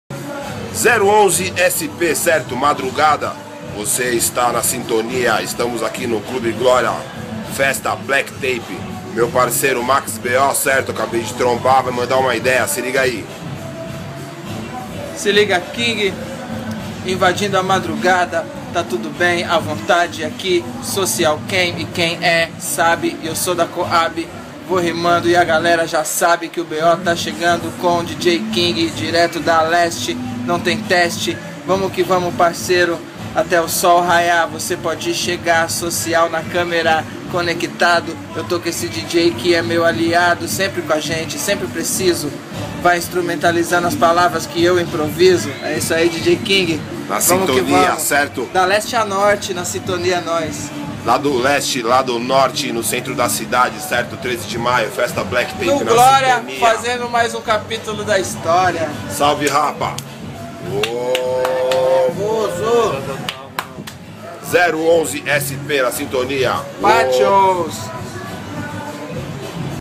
DJ KING £ MAX B.O. - 12/03/2012 - Black Tape - Madrugada... Role na Night !!!
Vídeo criado com o aplicativo Socialcam no iPhone.
Chamada do evento Black Tape / Clube Glória / SP, em 12/04/2012.